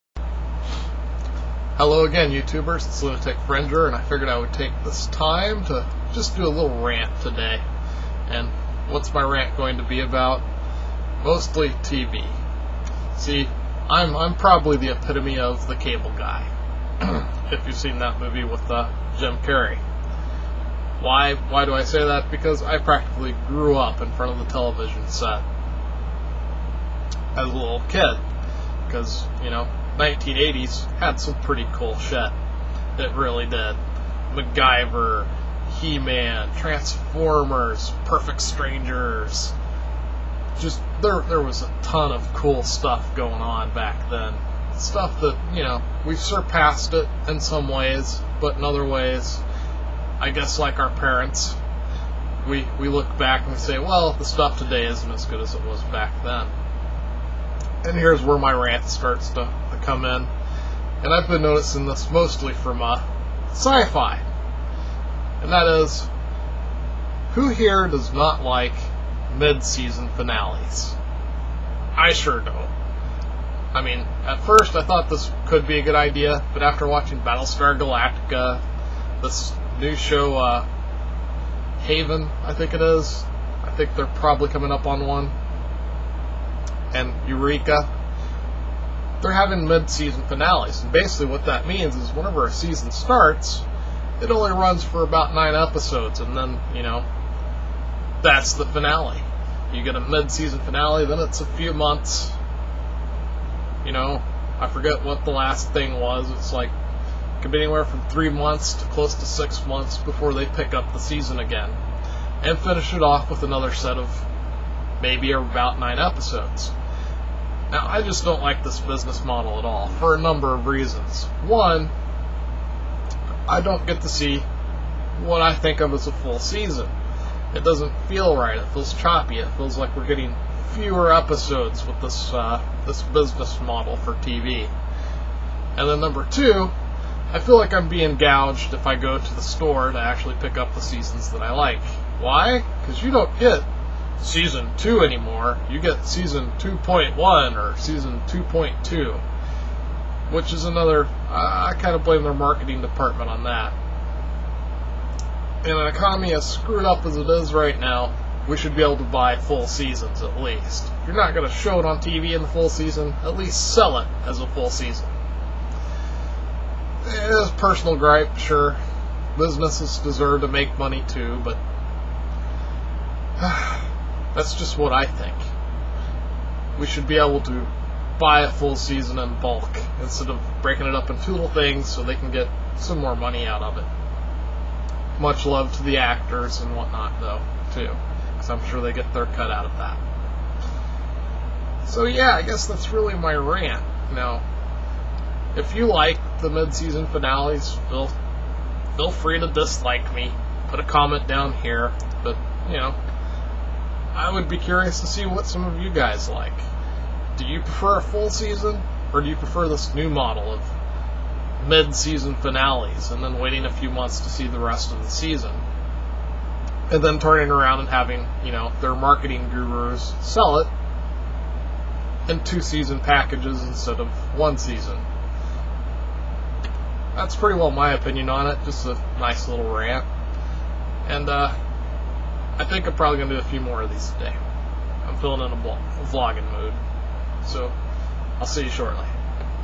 TV Seasons New Model Sucks To Me!
I weigh in on my opinion of the mid-season finale tv model.